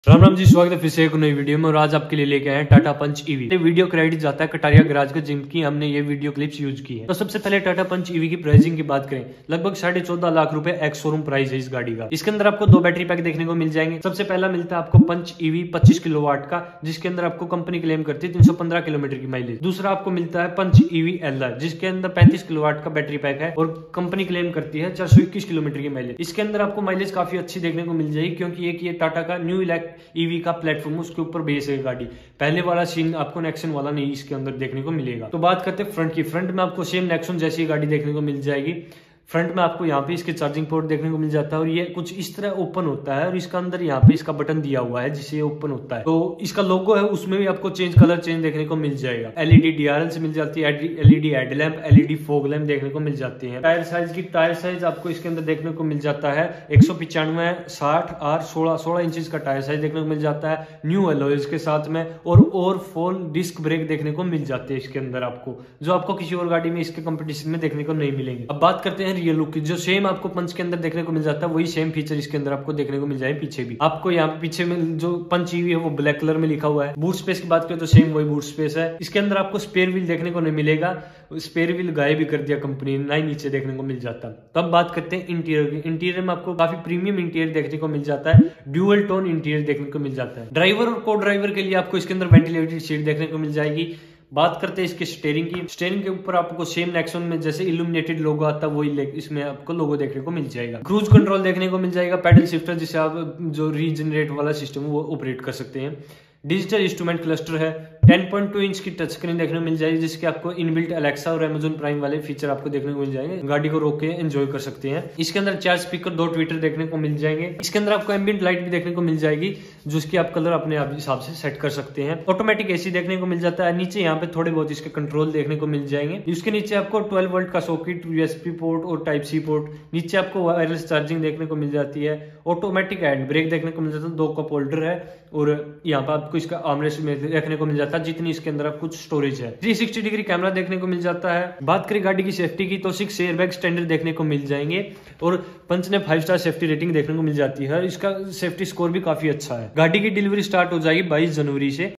2024 New TATA PUNCH EV |Tata की Electric Queen👍 फीचर में असली GAME CHANGER हिलाया Market
TATA PUNCH EV 2024 | ऐसे FEATURES मिलते है अब इसमें | हसली GAME CHANGER अब आ गई
FOLLOW US ON FACBOOK : DHANDA GARGE

CREDIT : KATARIA GARAGE

For Any Type Of Ownership Review